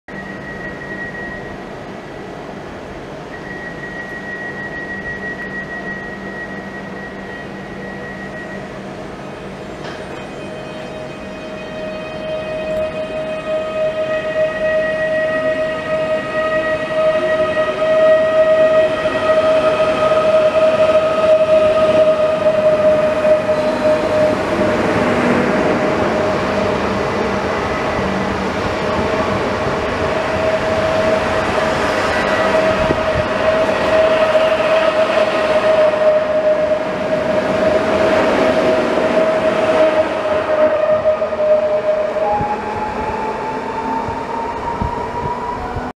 PKP Intercity #36 ED161-019 (IC3533 'Kochanowski' Rzeszów-Bydgoszcz)
ED161-019 ruszający ze stacji Rzeszów Główny jako IC3533 rel. Rzeszów Główny - Bydgoszcz Główna.
Sierpień 2022 r.